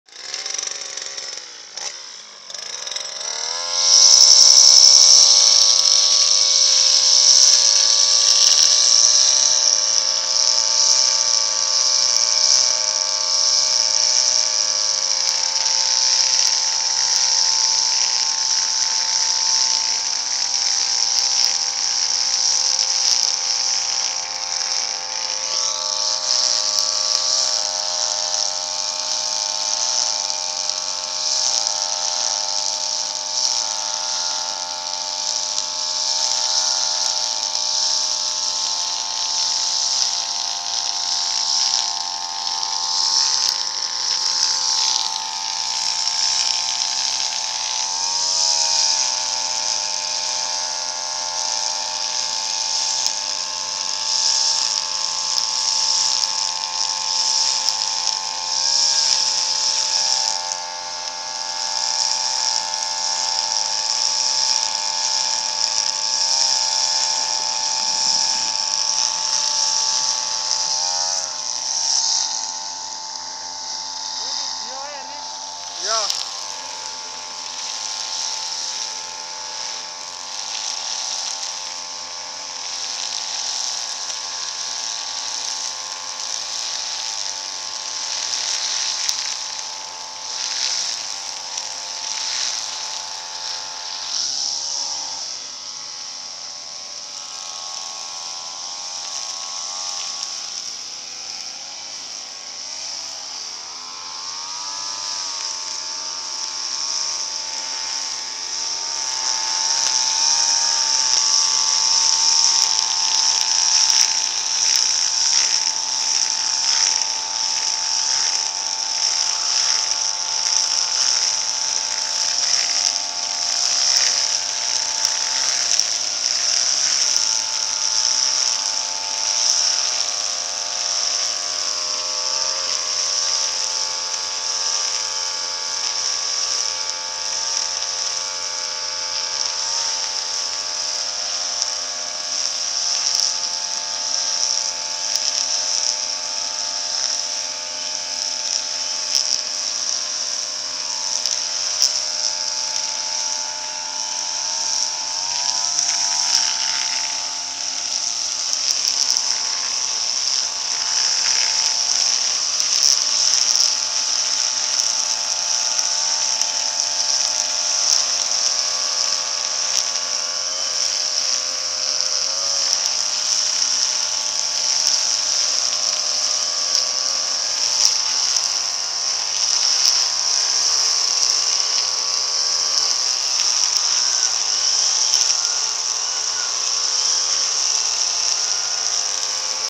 Motong rumput gaez
Motong rumput d tempat perkebunan
D lakukan setiap 1 bulan sekali.